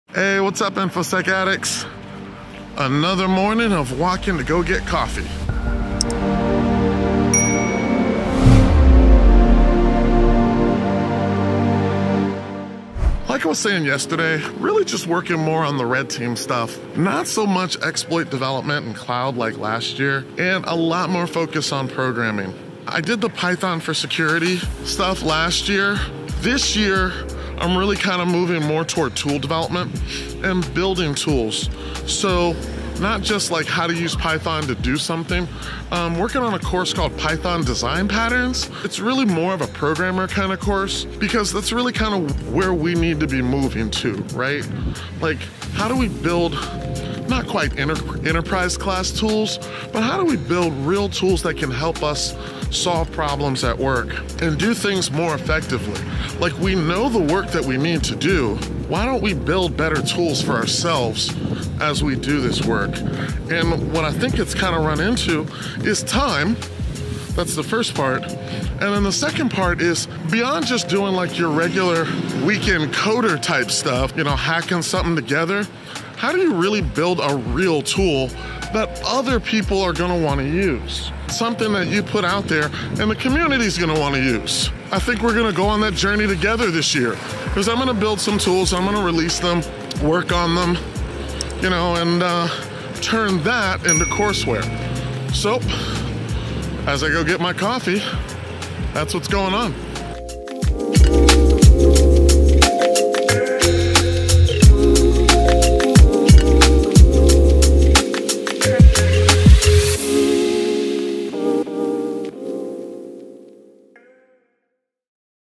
Python Design Patterns with Joe McCray | Mentor Moment 2021
Python Design Patterns with Joe McCray
In this Mentor Moment, What is a Design Pattern? Why Python?
A design pattern is a means by which a developer uses patterns more often to create software or web applications. It could also be defined simply as a common way of solving well-known problems. Python is a high-level language with dynamic binding and typing. It is powerful and many developers love and use python. Python is an object-oriented language (because everything in python is an object).